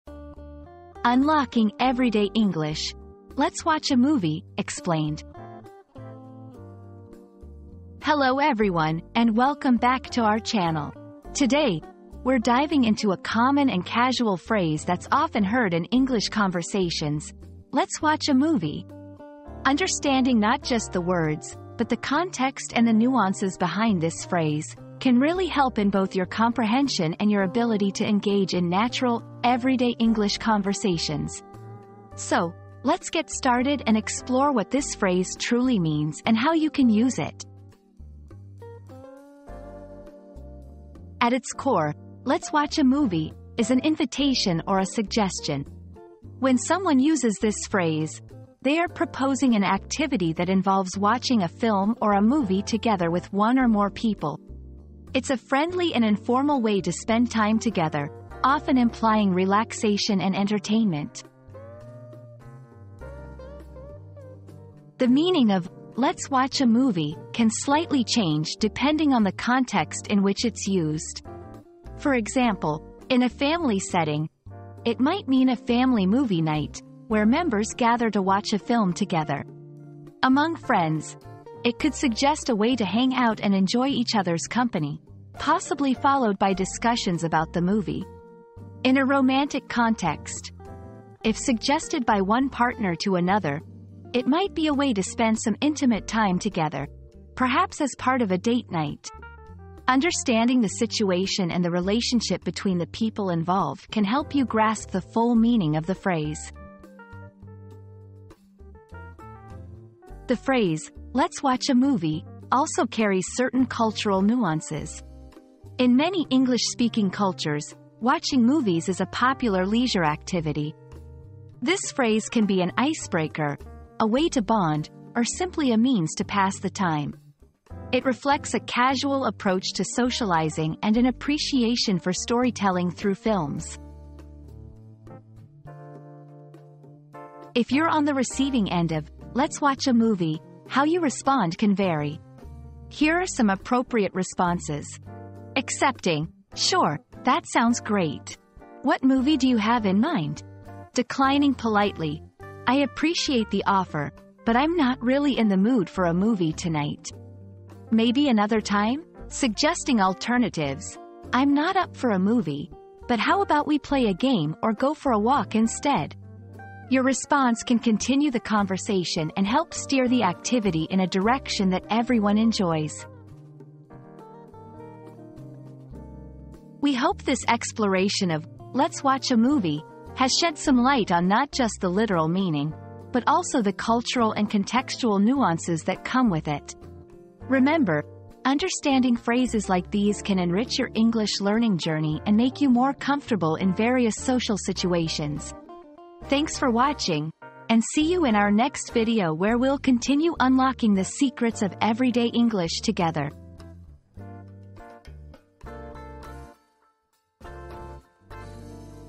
Unlocking Everyday English: "Let's Watch a Movie" Explained
Mastering Everyday English: Decoding 'Let's Watch a Movie' • Unravel the meaning behind the common phrase 'Let's Watch a Movie' and learn how to use it in everyday conversations. Watch now to enhance your English language skills!

00:00 • Introduction - Unlocking Everyday English: "Let's Watch a Movie" Explained
00:40 • The Literal Meaning
01:04 • The Context Matters
01:49 • Cultural Nuances
02:17 • Responding to the Invitation